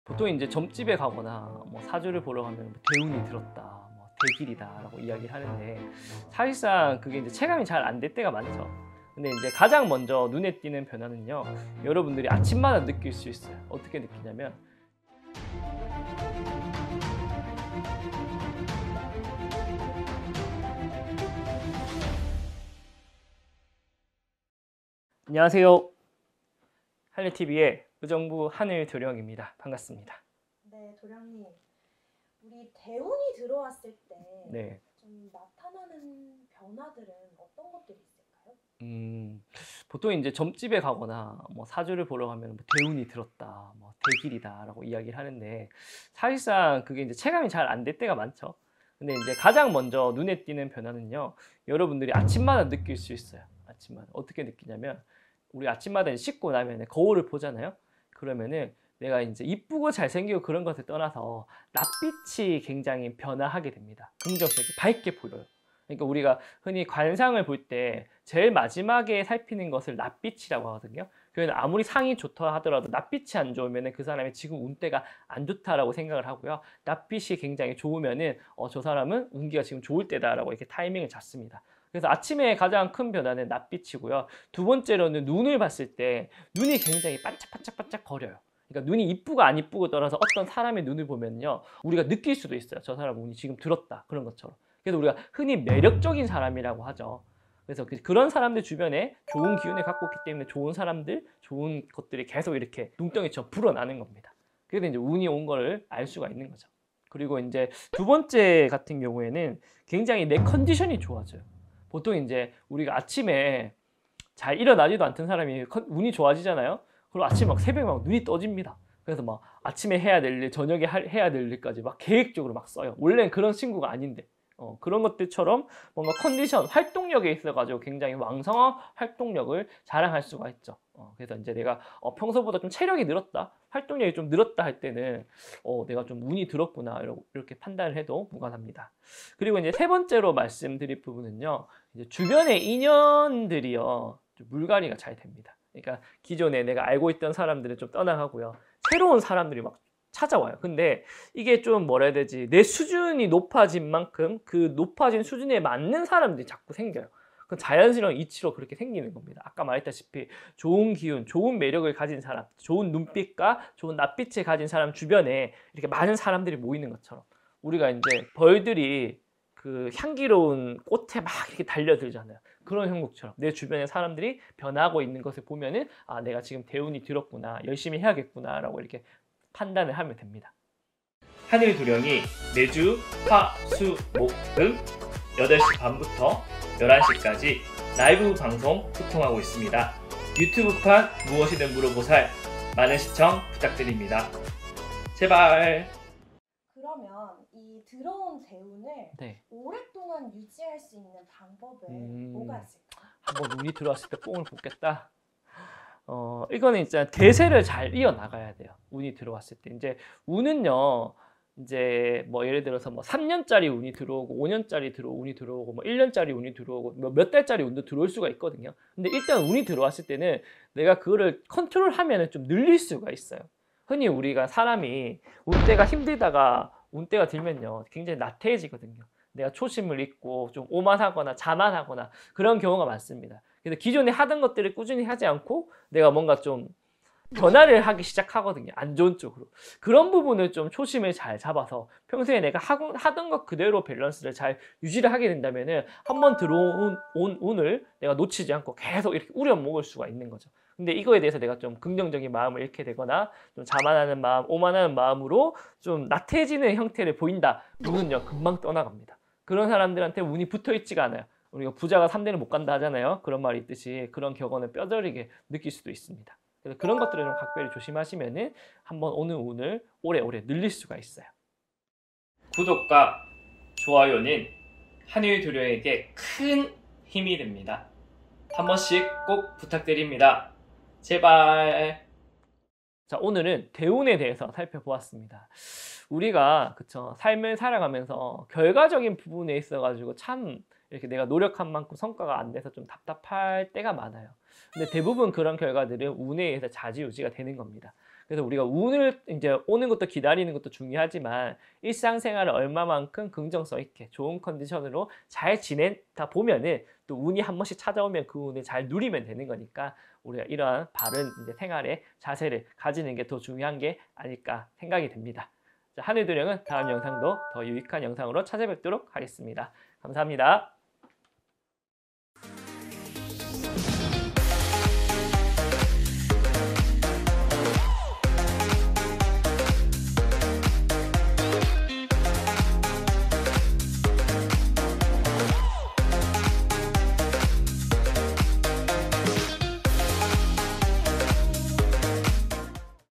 대운이 들어올 때 나타나는 3가지 징조! / 운이 좋아지는 신호 꼭 확인하세요!
대운이 들었을 때
나타나는 3가지 신호와 특징!
오래 유지하는 방법까지
✔지금 영상에서 확인하세요!